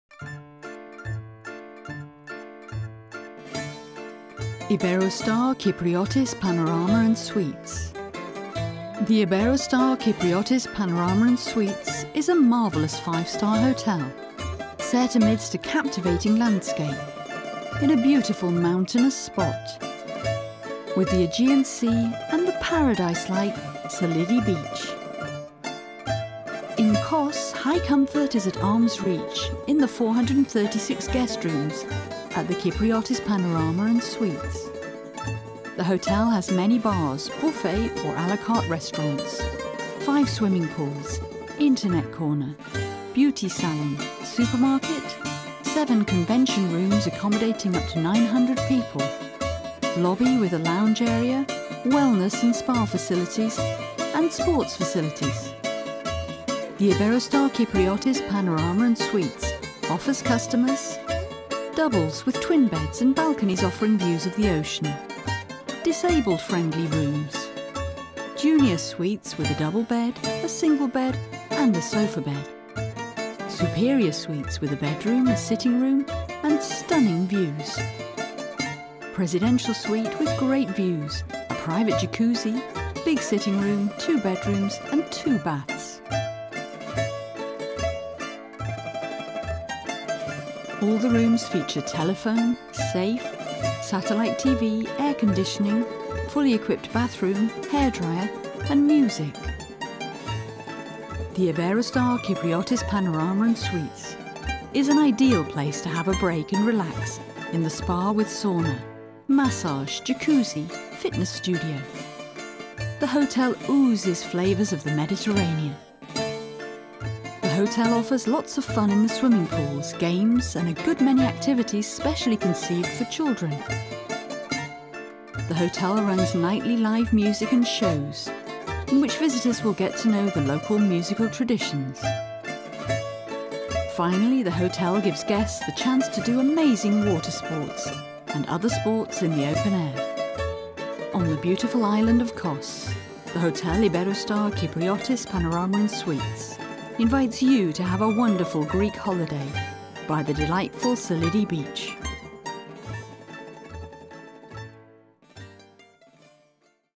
Kipriotis INGLES.mov
On the beautiful island of Kos the Hotel Iberostar Panorama & Suites invites you to spend a wonderful holiday in Greece overlooking the beautiful beach of Psalidi. Our fantastic 5-star hotel, surrounded by a truly enchanting landscape, is located in a delightful spot which blends its mountain setting with the presence of the Aegean Sea and the picture-postcard beach of Psalidi.